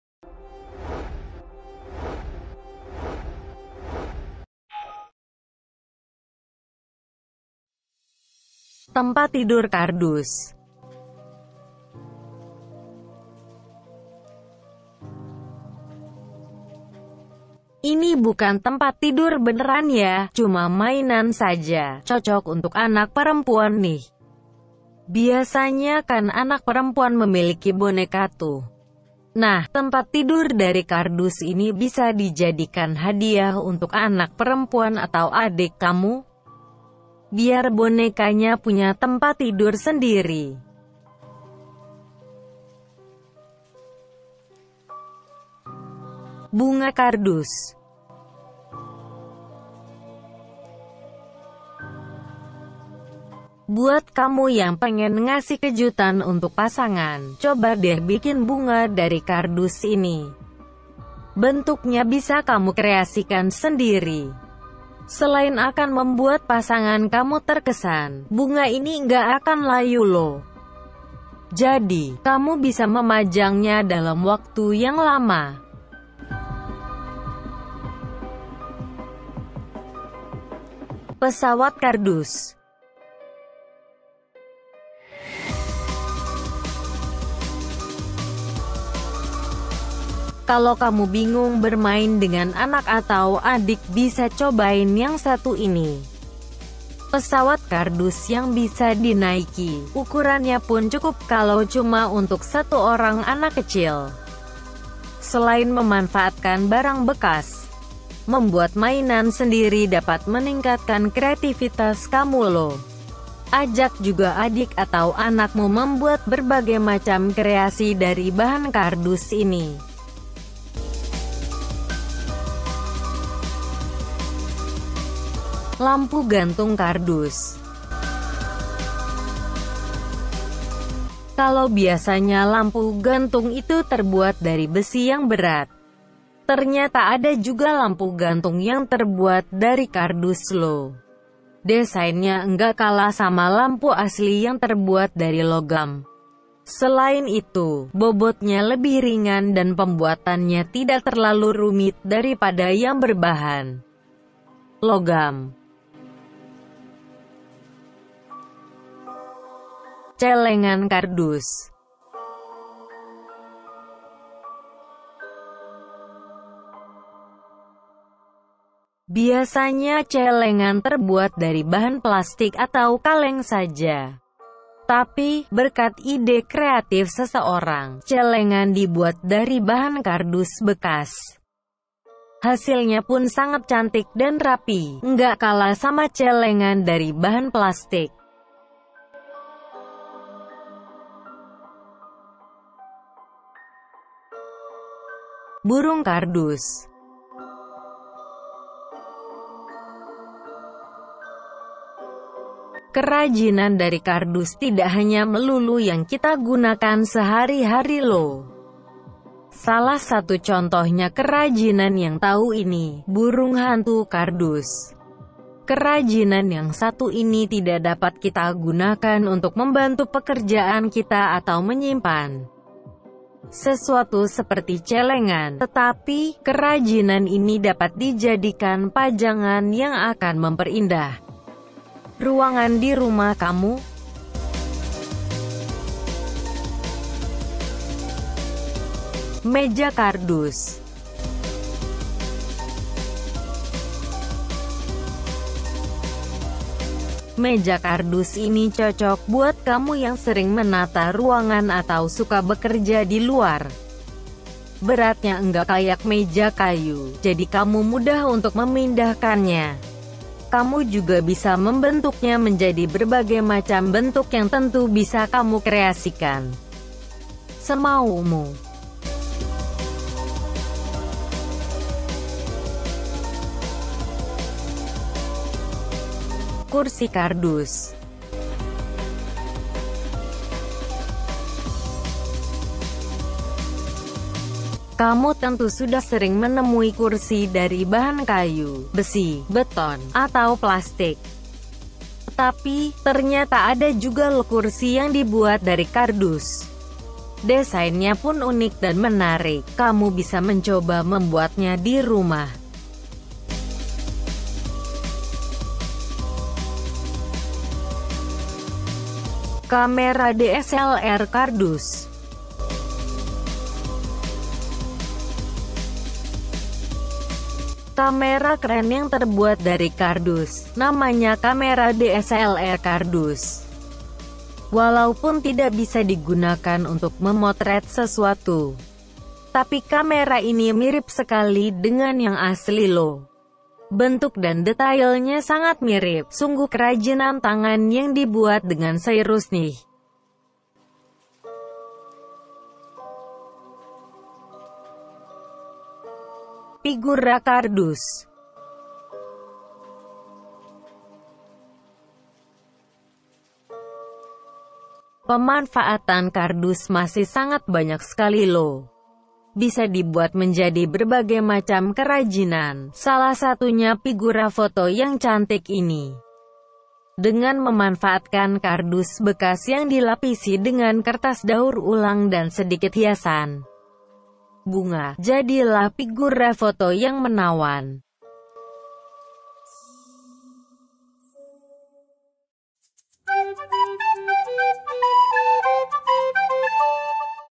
Kerajinan Tangan Dari Kardus, Aneka Kreasi Kardus
KURSI KARDUS
Kamu tentu sudah sering menemui kursi dari bahan kayu, besi, beton, atau plastik. Tapi, ternyata ada juga lho kursi yang dibuat dari kardus. Desainnya pun unik dan menarik, kamu bisa mencoba membuatnya di rumah.

MEJA KARDUS
Meja kardus ini cocok buat kamu yang sering menata ruangan atau suka bekerja di luar. Beratnya nggak kayak meja kayu, jadi kamu mudah untuk memindahkannya. Kamu juga bisa membentuknya menjadi berbagai macam bentuk yang tentu bisa kamu kreasikan semaumu.

CELENGAN KARDUS
Biasanya celengan terbuat dari bahan plastik atau kaleng saja. Tetapi, berkat ide kreatif seseorang, celengan dibuat dari bahan kardus bekas. Hasilnya pun sangat cantik dan rapi, nggak kalah sama celengan dari bahan plastik.

PIGURA KARDUS
Pemanfaatan kardus masih sangat banyak sekali lho, bisa dibuat menjadi berbagai macam kerajinan. Salah satunya pigura foto yang cantik ini. Dengan memanfaatkan kardus bekas yang dilapisi dengan kertas daur ulang dan sedikit hiasan bunga, jadilah pigura foto yang menawan.

BUNGA KARDUS
Buat kamu yang pengen ngasih kejutan untuk pasangan, coba deh bikin bunga dari kardus ini. Bentuknya bisa kamu kreasikan sendiri. Selain akan membuat pasangan kamu terkesan, bunga ini nggak akan layu lho. Jadi, kamu bisa memajangnya dalam waktu yang lama.

PESAWAT KARDUS
Kalau kamu bingung bermain dengan anak atau adik, bisa cobain yang satu ini, pesawat kardus yang bisa dinaiki. Ukurannya pun cukup kalau cuma untuk satu orang anak kecil.Selain memanfaatkan barang bekas, membuat mainan sendiri dapat meningkatkan kreatifitas kamu lho. Ajak juga adik atau anakmu membuat berbagai macam kreasi dari bahan kardus ini.

BURUNG KARDUS
Kerajinan dari kardus tidak hanya melulu yang kita gunakan sehari-hari lho. Salah satu contohnya kerajinan yang tau ini, burung hantu kardus.Kerajinan yang satu ini tidak dapat kita gunakan untuk membantu pekerjaan kita atau menyimpan sesuatu seperti celengan. Tapi, kerajinan ini dapat dijadikan pajangan yang akan memperindah ruangan di rumah kamu.

TEMPAT TIDUR KARDUS
Ini bukan tempat tidur beneran ya, cuma mainan saja, cocok untuk anak perempuan nih. Biasanya kan anak perempuan memiliki boneka tuh. Nah, tempat tidur dari kardus ini bisa dijadikan hadiah untuk anak perempuan atau adik kamu. Biar bonekanya punya tempat tidur sendiri.

LAMPU GANTUNG KARDUS
Kalau biasanya lampu gantung itu terbuat dari besi yang berat. Ternyata ada juga lampu gantung yang terbuat dari kardus lho. Desainnya nggak kalah sama lampu asli yang terbuat dari logam. Selain itu, bobotnya lebih ringan dan pembuatannya tidak terlalu rumit dari pada yang berbahan logam.

KAMERA DSLR KARDUS
Kamera keren yang terbuat dari kardus, namanya kamera DSLR kardus. Walaupun tidak bisa digunakan untuk memotret sesuatu, tapi kamera ini mirip sekali dengan yang asli lho. Bentuk dan detailnya sangat mirip, sungguh kerajinan tangan yang dibuat dengan seirus nih.